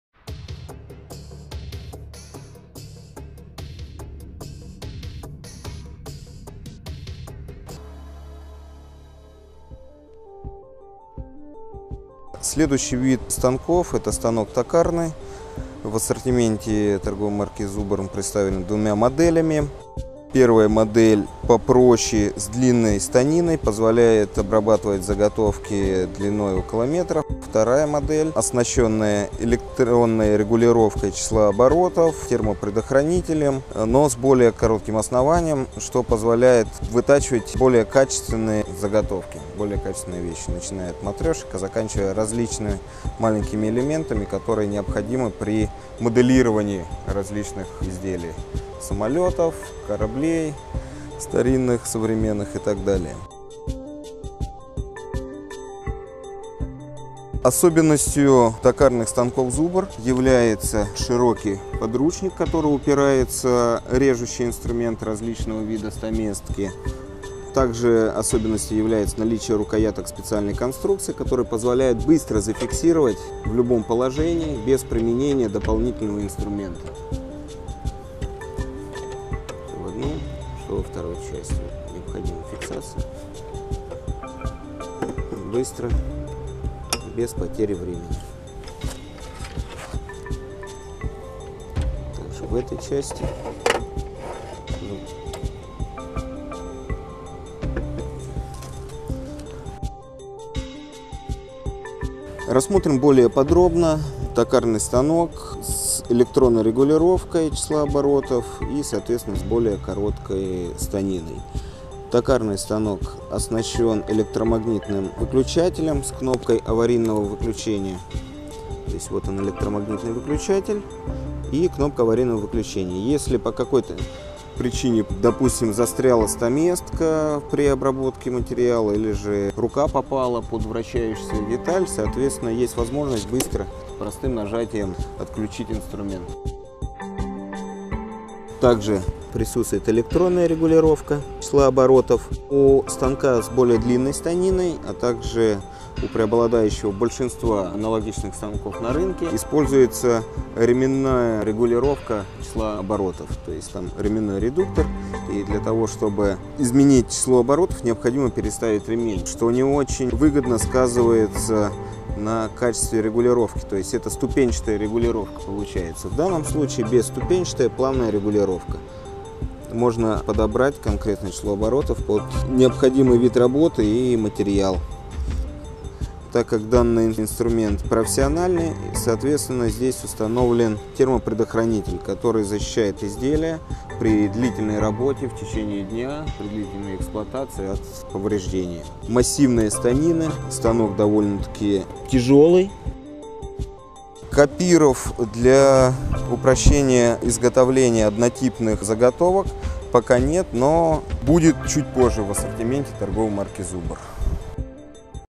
Обзор Токарные станки по дереву ЗУБР ЗСТД-350-1000 и ЗУБР ЗСТД-350-330
Токарный станок ЗУБР ЗСТД-350-1000 — используется для вытачивания цилиндрических заготовок. Модель оснащена магнитным выключателем для предотвращения случайного включения после внезапного отключения электроэнергии. Максимальная длина обрабатываемого изделия — 1000 мм. Частота вращения регулируется — от 810 до 2480 оборотов в минуту.

Токарный мини-станок ЗУБР ЗСТД-350-330 — предназначен для вытачивания цилиндрических заготовок из дерева. Модель отличается электронной регулировкой оборотов, простой установкой и прочной металлической конструкцией. Длинная станина позволяет вытачивать практически любые детали (даже очень мелкие). Двигатель мощностью 350 Вт не вызывает перегрузок в сети.